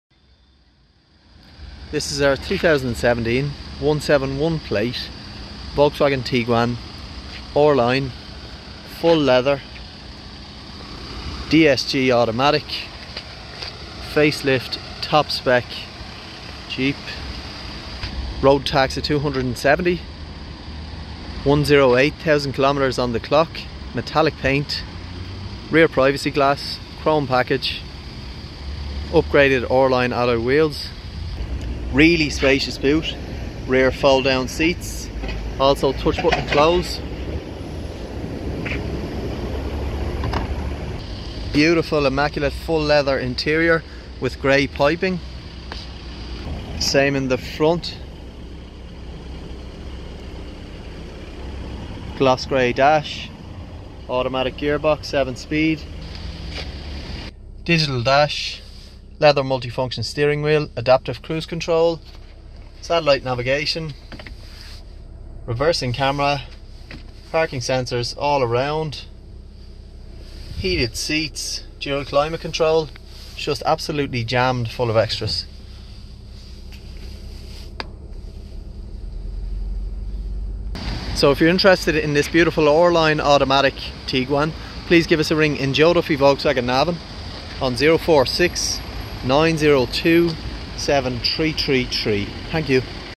171KE1128 - 2017 Volkswagen Tiguan R-LINE HL 2.0TDI 150HP D7F 5DR 32,950
Here is a video of our 171KE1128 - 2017 Volkswagen Tiguan R-LINE HL 2.0TDI 150HP D7F 5DR 32,950. Please contact SALESMAN NAME to arrange a test drive today. Joe Duffy Volkswagen NavanPhone: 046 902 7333